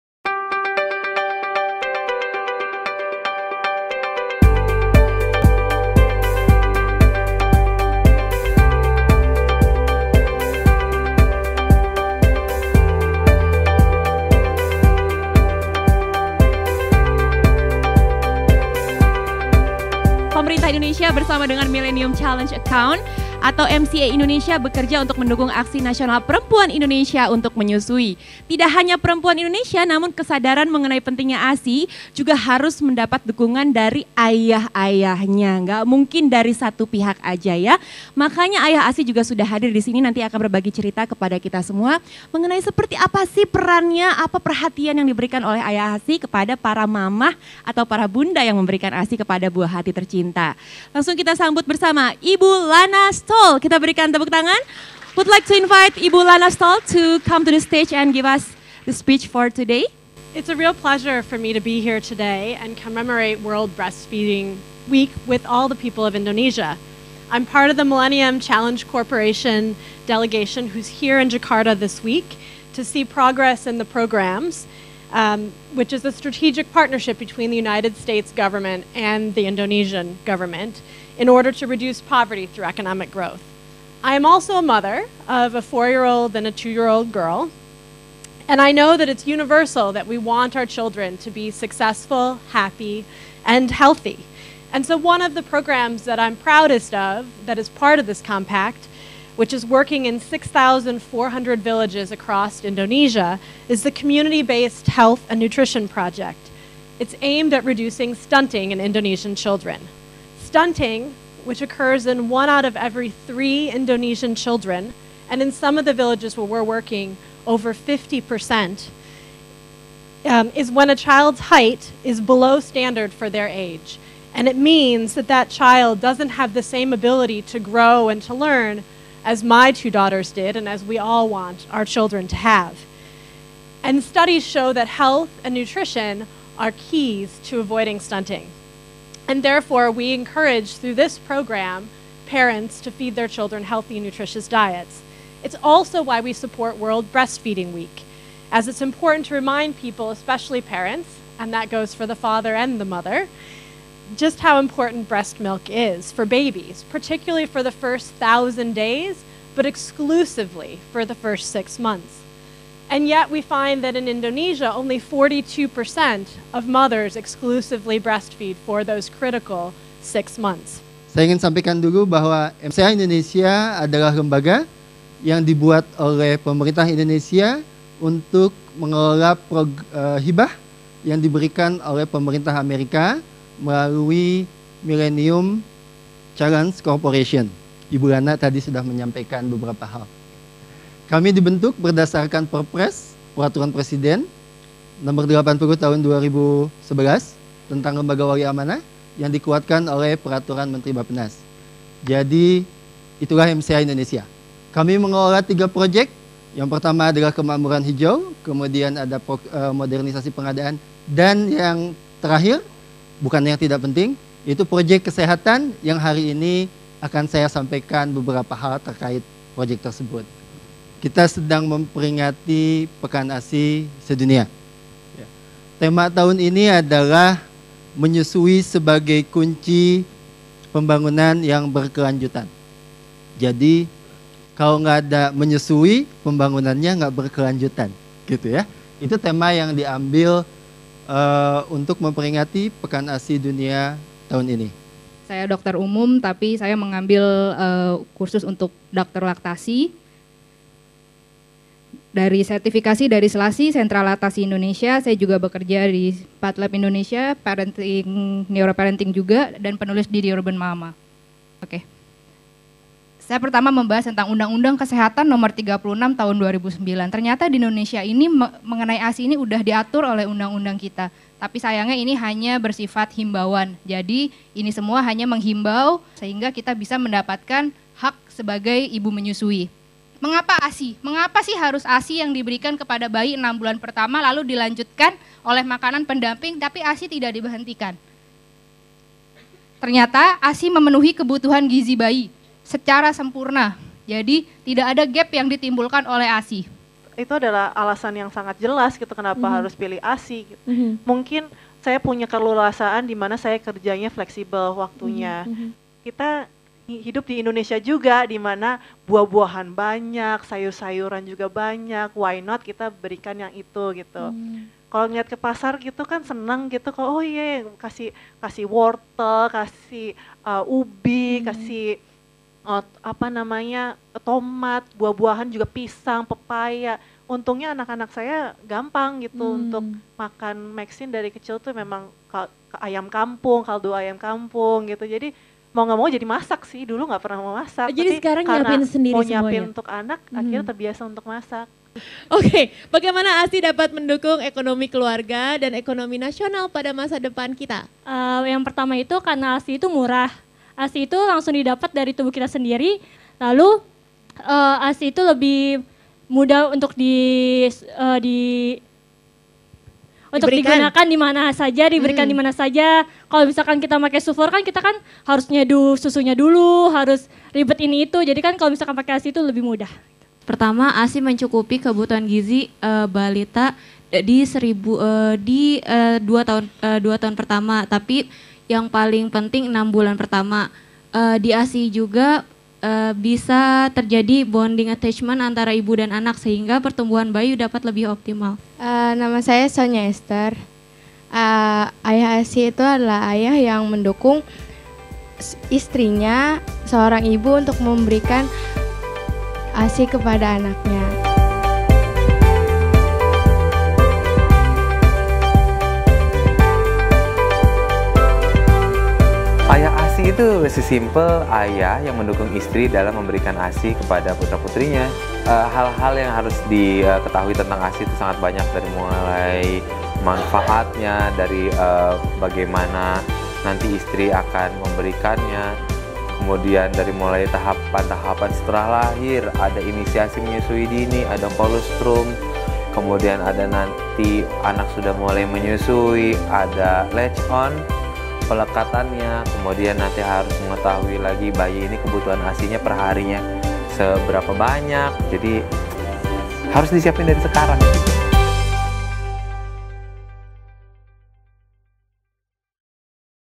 Talkshow "Keajaiban Menyusui"
Komunitas pegiat ASI, mahasiswa yang bergelut di bidang kesehatan dan gizi, jurnalis, serta masyarakat umum dengan antusias memenuhi kursi audiens di studio @america pada 3 Agustus 2016. Mereka hadir untuk mengikuti acara bincang-bincang bertajuk Keajaiban Menyusui yang diselenggarakan oleh MCA-Indonesia untuk memperingati Pekan Menyusui Sedunia. Apa yang mereka bicarakan? Simak videonya di sini, dan baca artikelnya di

Breastfeeding advocates, students of health and nutrition, journalists, and general public enthusiastically packed @america's auditorium on August 3, 2016. They actively participated in the "Miracle of Breastfeeding" talkshow, which was held by MCA-Indonesia to commemorate World Breastfeeding Week. What did they talk about? Check out the video here and read the article in